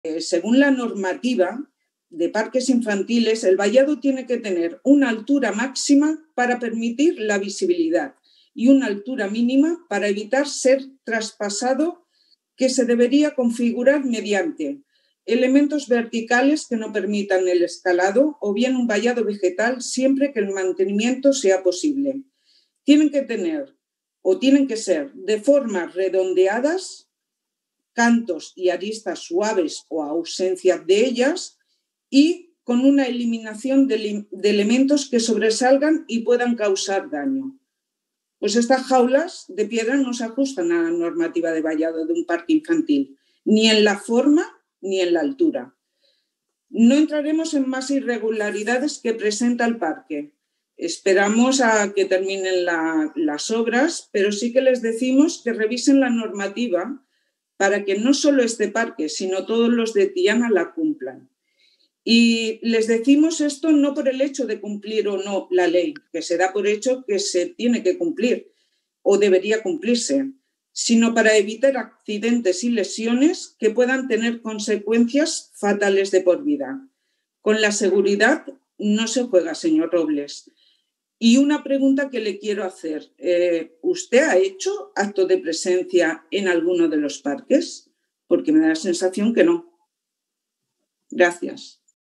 Seguretat als parcs infantils
Intervenció de la regidora Núria Amado al ple de febrer del 2021 sobre el manteniment i seguretat als parcs.